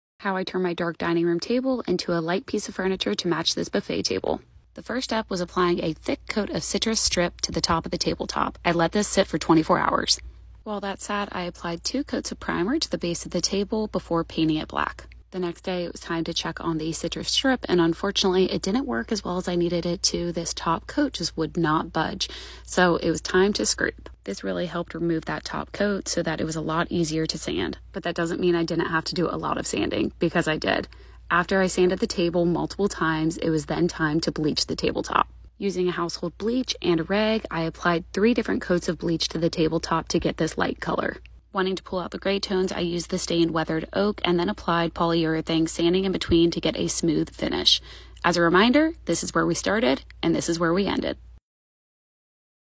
Bleaching my Dining Room Table
A little citrus strip, a lot of sanding, a bit of bleaching and a tad of stain has this table looking completely different!